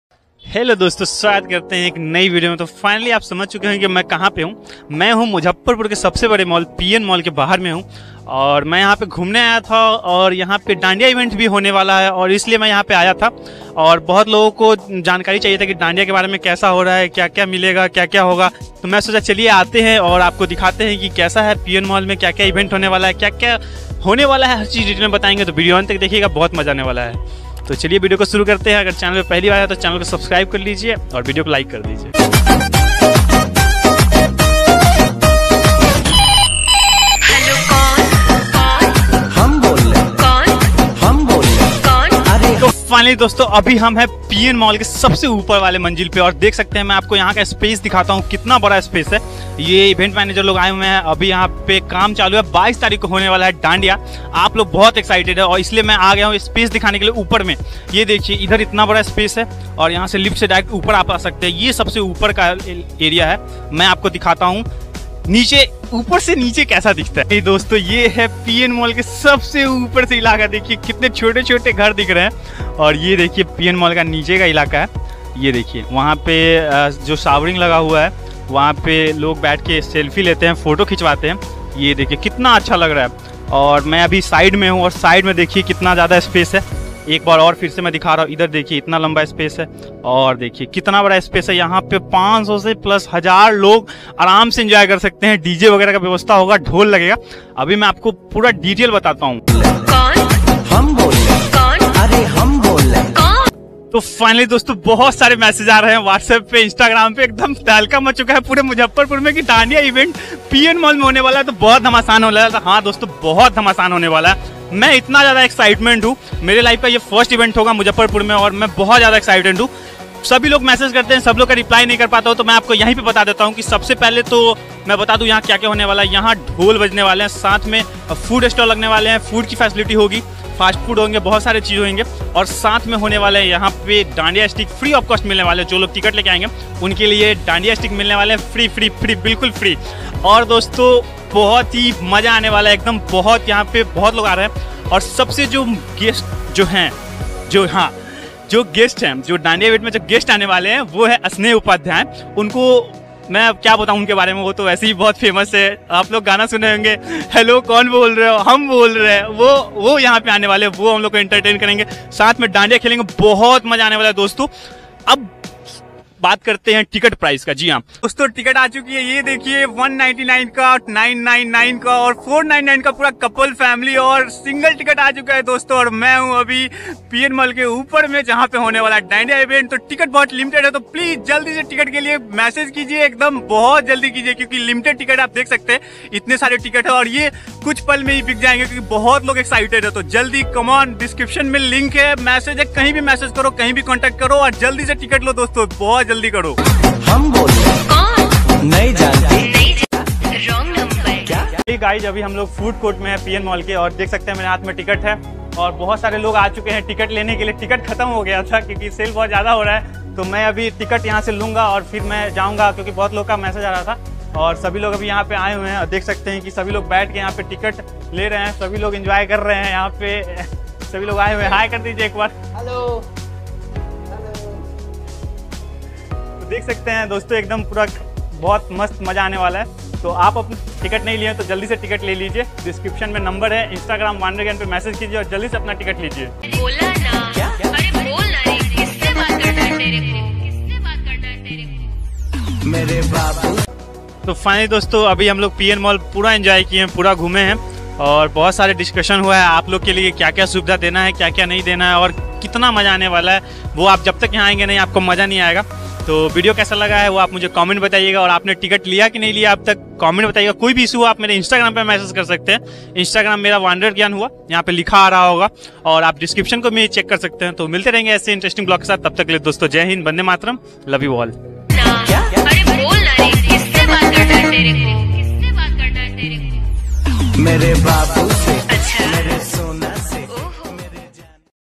Dandiya Night 2023 in P&M Mall Muzaffarpur with @SnehUpadhya😍 | Wanderer Gyan
Details - MUZAFFARPUR GRAND DANDIYA NIGHT 🔥2K23 ✅
WE ARE BACK ⚡
READY TO WELCOME YOU !!
DATE - 22th / OCT / 2023
VENUE - P&M MALL MUZAFFARPUR

➡️ ARE YOU GUYS READY 😉🎉

EXPERIENCE -
• LIVE DHOL
• FOOD STALLS
• FREE DANDIYA STICKS & LOTS OF MORE !!

➡️ PRICES -
• FOR SINGLE - ₹199 ( ONLY FOR FEMALES )
• FOR COUPLES - ₹499
• FOR FAMILY - ₹999 [ UPTO 5 PEOPLE'S / MEMBER'S ]

➡️ FOLLOW FOR MORE UPDATES , ON IG - wanderer_gyan
HURRY !! PRE - BOOK YOUR PASSES NOW ✅

➡️ BE A PART OF MUZAFFARPUR GRAND DANDIYA NIGHT ⚡ WITH SNEH UPADHYAY

REGARDS FROM -
" WANDERER GYAN"